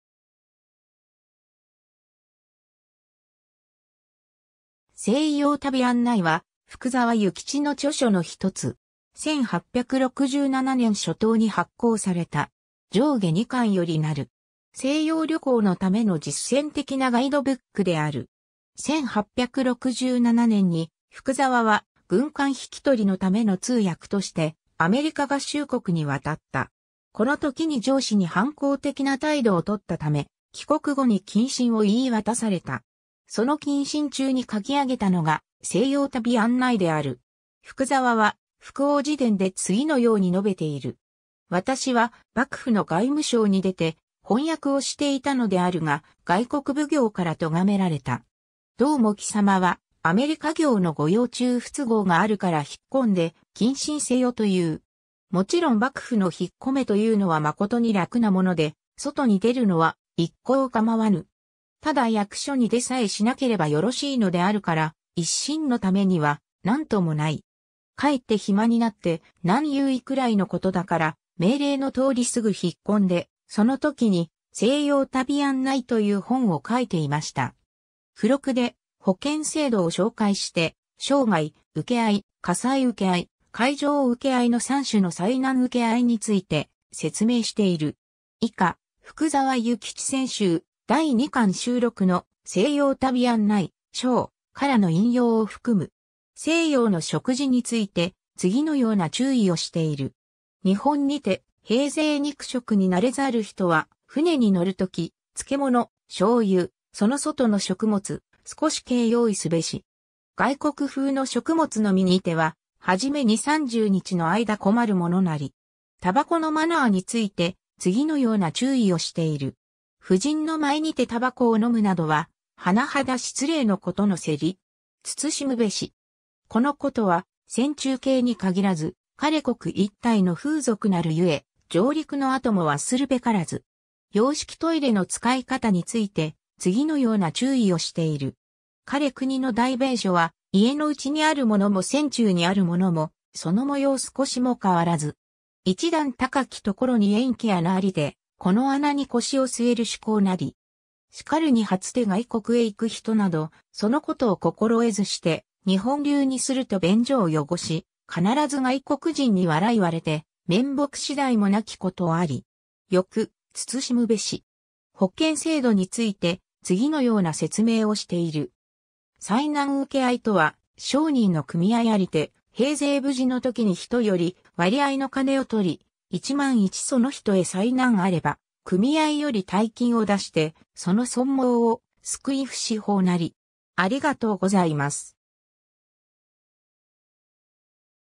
西洋旅案内
『西洋旅案内』（せいようたびあんない）は、福澤諭吉の著書のひとつ。
1867年（慶応3年）初冬に発行された。
上下2巻よりなる。
西洋旅行のための実践的なガイドブックである。
 1867年（慶応3年）に福澤は軍艦引き取りのための通訳としてアメリカ合衆国に渡った。
この時に上司に反抗的な態度を取ったため、帰国後に謹慎を言い渡された。
その謹慎中に書き上げたのが『西洋旅案内』である。
 福澤は『福翁自伝』で次のように述べている。
 私は幕府の外務省に出て翻訳をしていたのであるが、外国奉行から咎（とが）められた。
「ドウも貴様はアメリカ行の御用中不都合があるから引っ込んで謹慎せよ」と言う。
勿論（もちろん）幕府の引っ込めというのは誠に楽なもので、外に出るのは一向構わぬ。
ただ役所に出さえしなければ宜（よろ）しいのであるから、一身のためには何（なん）ともない。
却って暇になって難有（ありがた）いくらいのことだから、命令の通り直ぐ引っ込んで、その時に西洋旅案内という本を書いていました。
 附録で保険制度を紹介して、生涯請合（生命保険）、火災請合（火災保険）、海上請合（損害保険）の3種の災難請合について説明している。
 以下、『福沢諭吉選集〈第2巻〉』（岩波書店）収録の「西洋旅案内〔抄〕」からの引用を含む。
 西洋の食事について、次のような注意をしている。
 日本にて平生肉食（にくじき）に馴れざる人は、船に乗るとき、漬物、醤油、其外の食物（しよくもつ）、少し計（ばかり）用意すべし。
外国風の食物のみにては、はじめ二、三十日の間（あいだ）困るものなり。
 タバコのマナーについて、次のような注意をしている。
 婦人の前にて烟草をのむなどは、甚だ失礼のことゝせり。
謹（つつし）むべし。
このことは船中計（ばかり）に限らず、彼国（かのくに）一体の風俗なるゆへ、上陸の後も忘るべからず。
 洋式トイレの使い方について、次のような注意をしている。
 彼国の大便所は、家の内にあるものも船中にあるものも、其模様少しも替らず。
一段高き所に円（まろ）き穴ありて、この穴に腰を据（すえ）る趣向（しゆこう）なり。
然（しか）るに初（はじめ）て外国へ行く人など、其ことを心得ずして、日本流にすると便所を汚（けが）し、かならず外国人に笑はれて、面目次第もなきことあり。
よく＼／つゝしむべし。
 保険制度について、次のような説明をしている。
 災難請合（さいなんうけあい）とは、商人の組合ありて、平生（へいぜい）無事の時に人より割合の金を取り、万一其人へ災難あれば組合より大金を出（いだ）して其（その）損亡を救ふ仕法（しほう）なり。